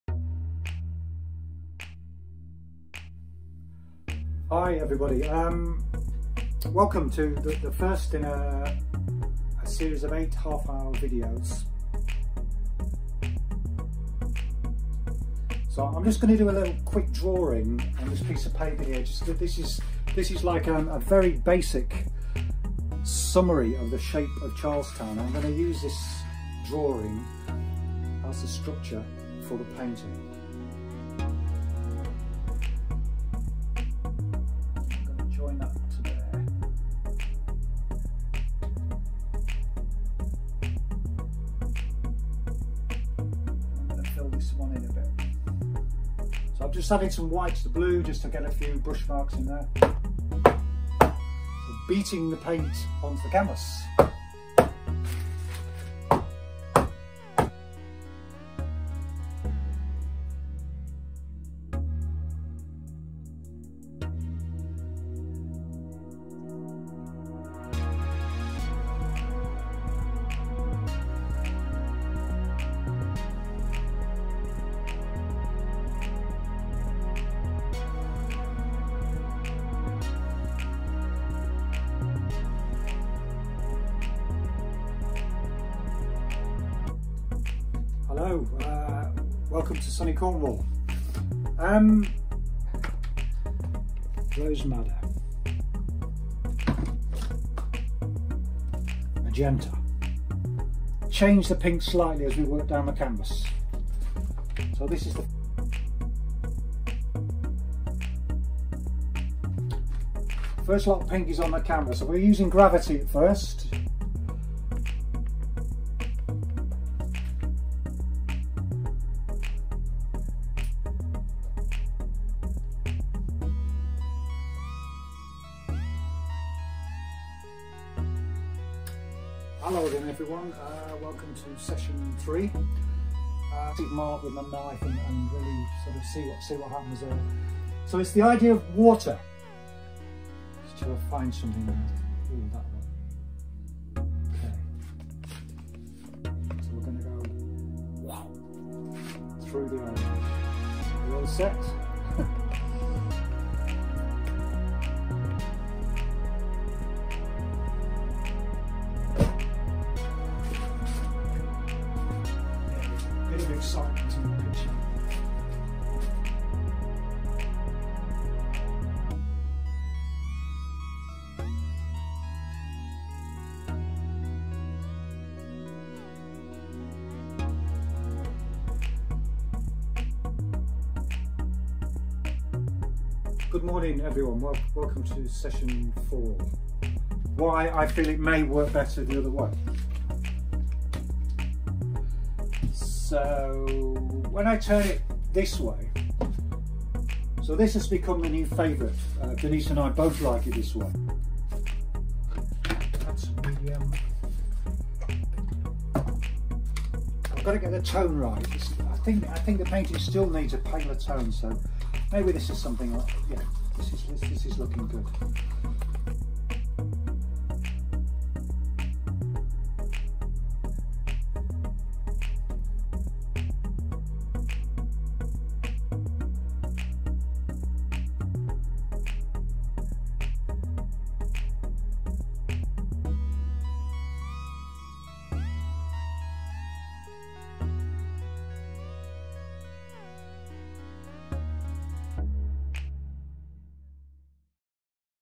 'The Painting Process '1 with Ashley Hanson (Highlights)
Renowned colourist Ashley Hanson gives you an insight into his ideas and process as he works on a new Cornish harbour painting from beginning to end.

There are 8 exclusive half hour sessions available where Ashley completes two very different harbour paintings, sharing his tips and techniques along the way.